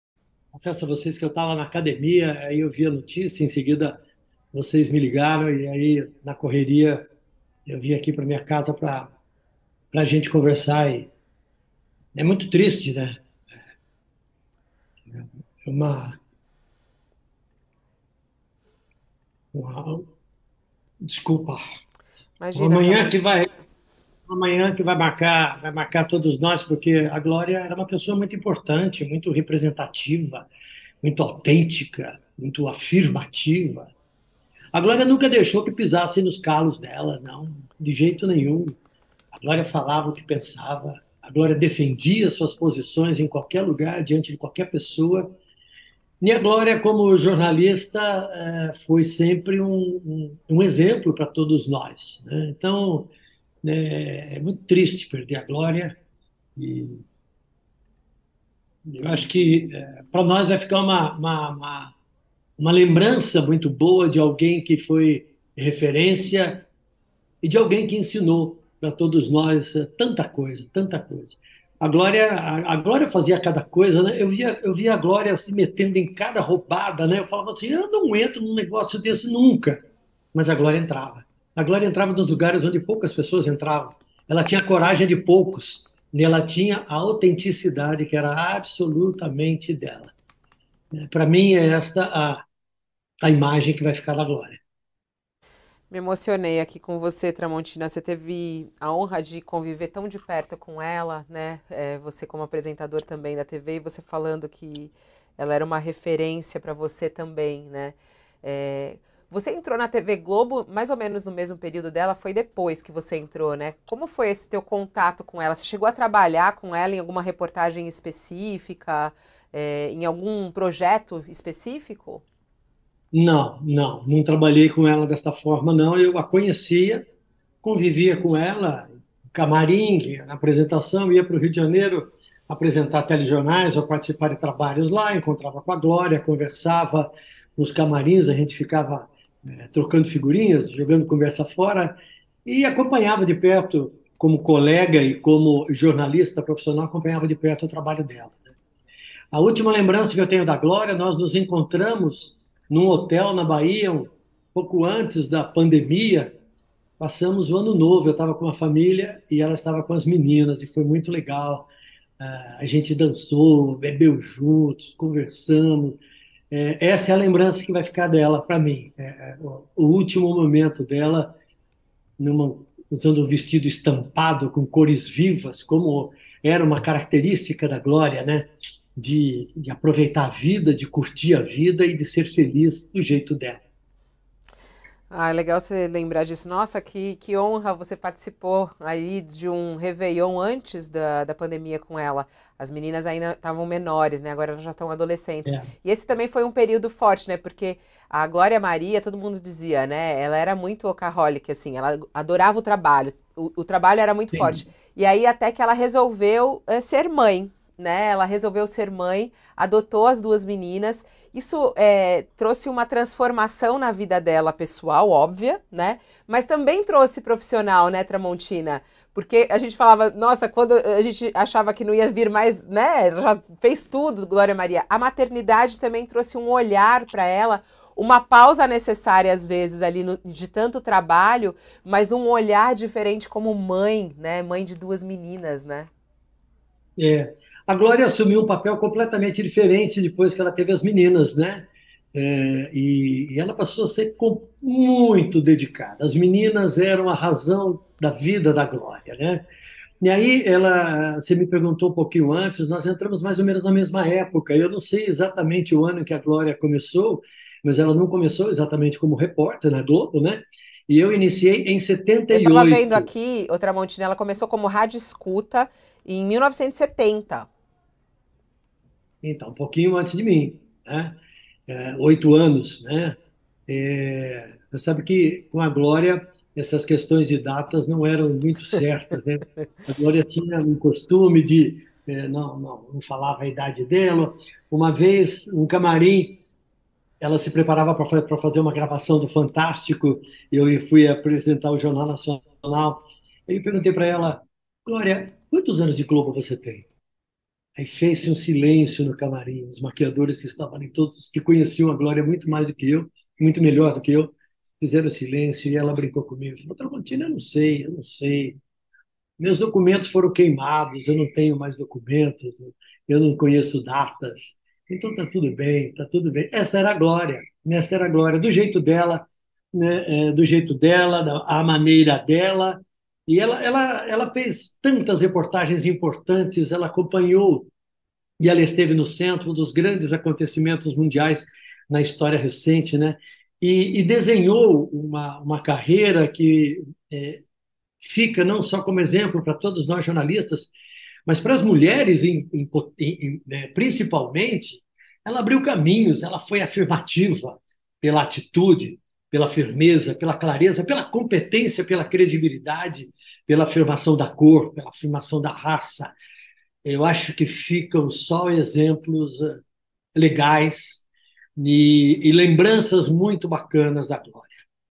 Morre Glória Maria: Tramontina se emociona ao falar da jornalista 'Foi referência e ensinou muito'
Com a morte de Glória Maria, nesta quinta (02/02), Carlos Tramontina compartilhou sua experiência com a jornalista e se emocionou ao falar dos momentos que viveram e os aprendizados.